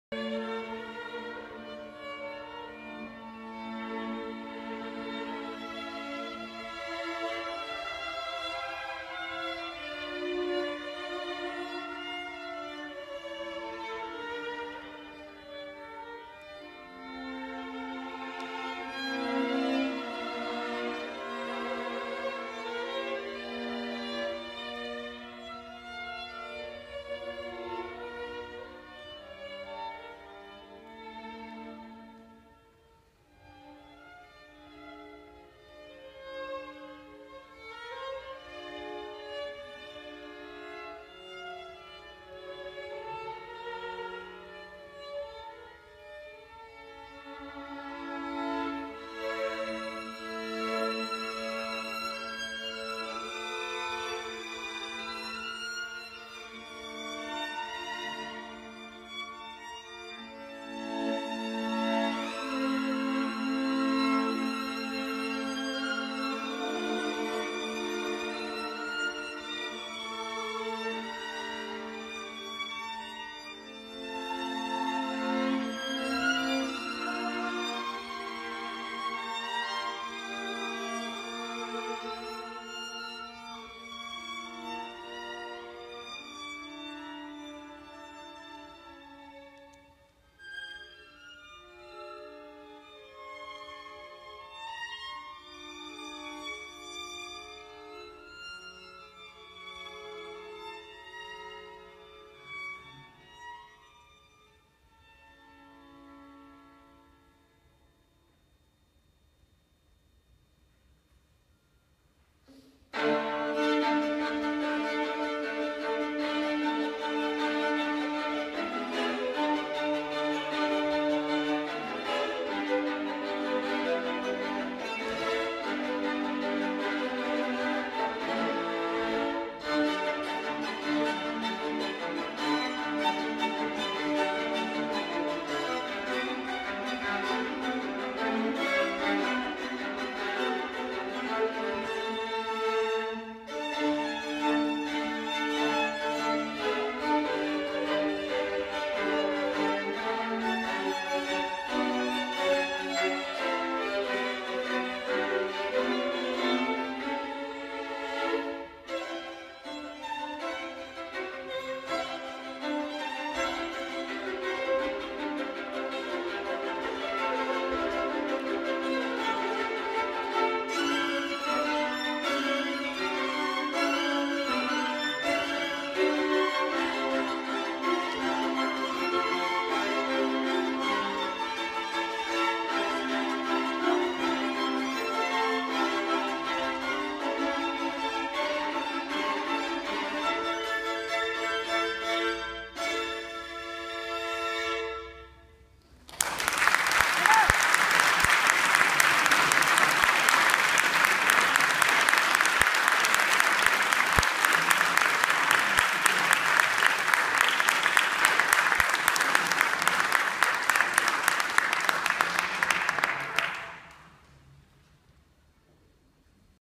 The Carpenter Trio
I think this is Bartok. At the Violin Museum in Cremona